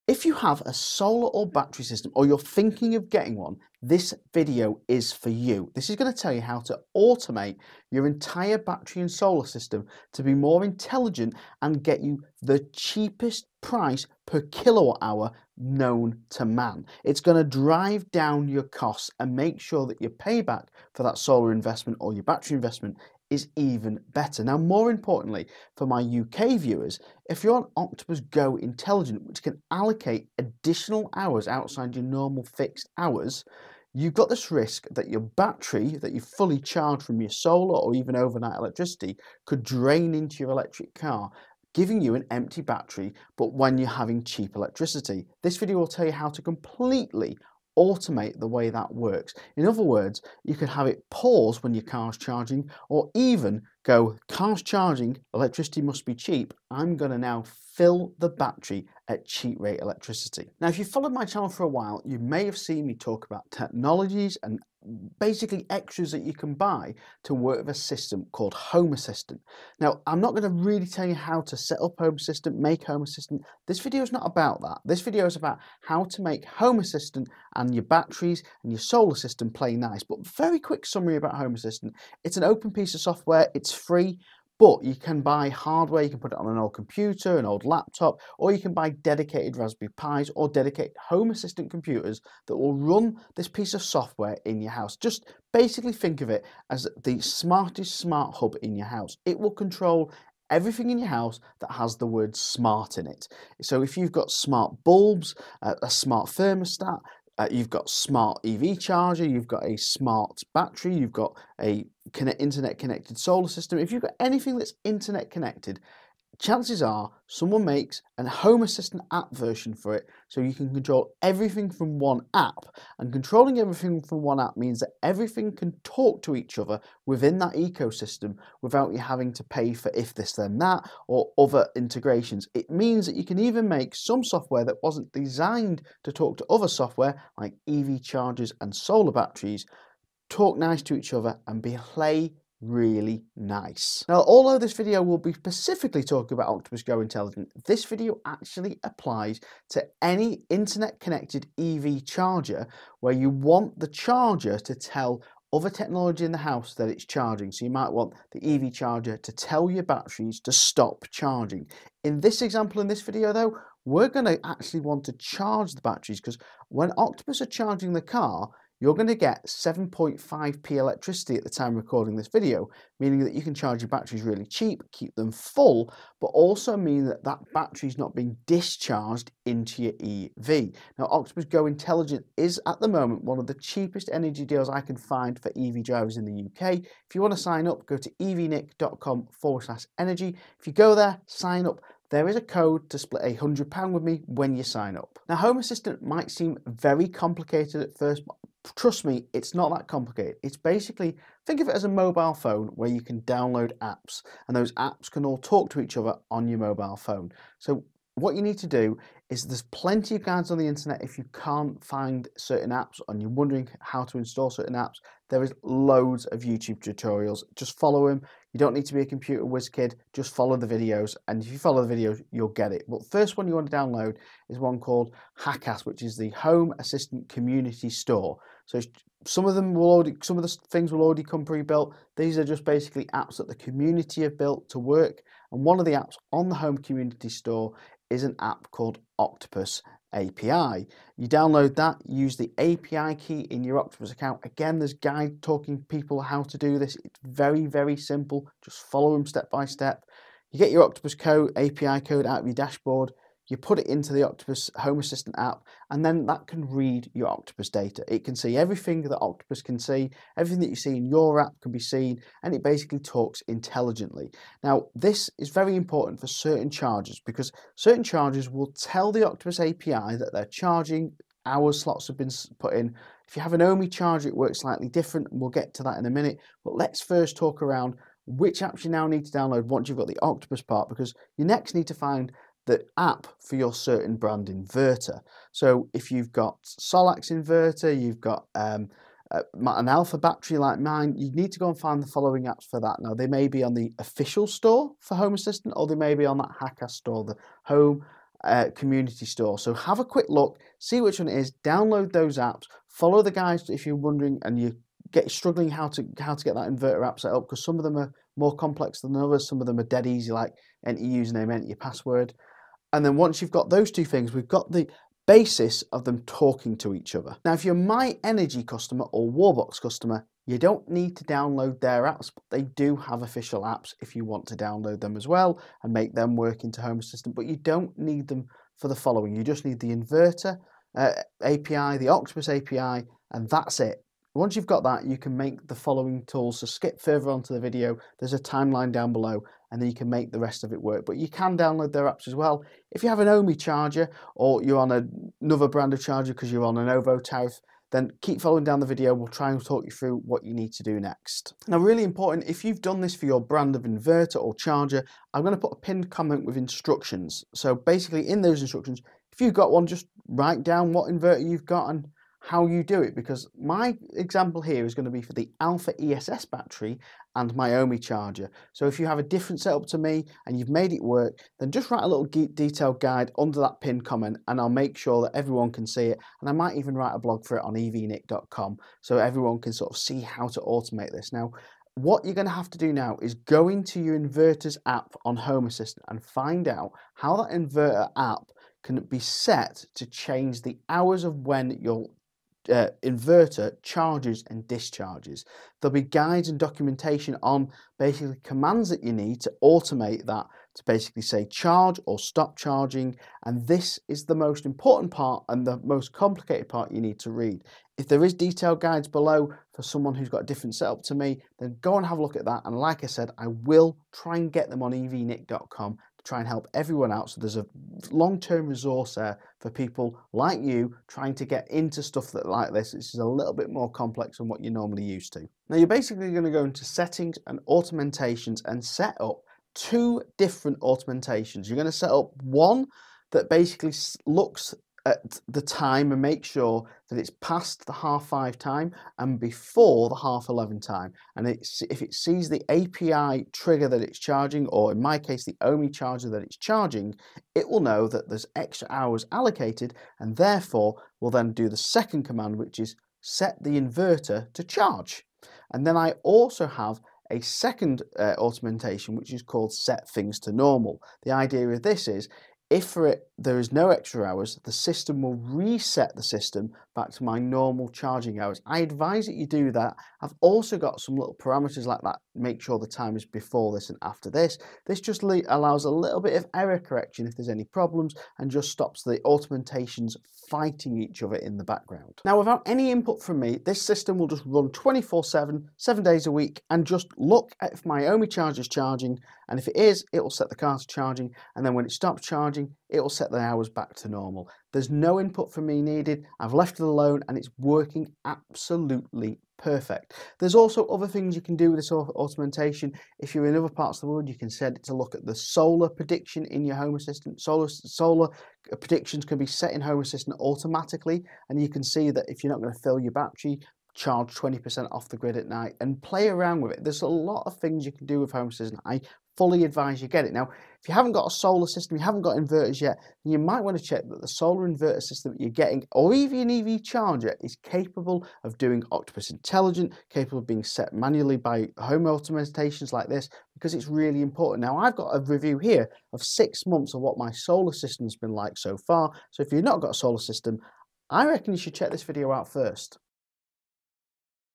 Make Your Battery Talk To Any EV Charger To Stop It Discharging!
Budget Way To Get ANY EV Charger Talk To Your Battery.
You don't need to buy the same brands EV chargers and battery systems to have them all talk,  if like me you have an adaptive energy deal that chargers your car randomly depending on electric costs and then are fighting to stop your home energy battery from discharging to the grid this video explains how you can truly make your SMART devices smart.  Have your EV charger tell your smart home its charging and to stop discharging the battery on deals like octopus intelligent and instead charge the battery during this cheap offpeak energy deal.   The uses for this are endless!

00:00 Solar & Battery System Automation
01:00 Home Assistant Solar And Battery System
02:24 EV Charger talk to home storage battery
03:15 Home Assistant Apps Your NEED
06:22 Setup Inverter settings
07:07 Setup Automation (see Pin'd Comments)
08:40 Not got solar yet?

£50 credit on your gas and electric octopus go 7.5p per kWh offpeak!  "straw-moon-586" to find the best octopus deal and my full code go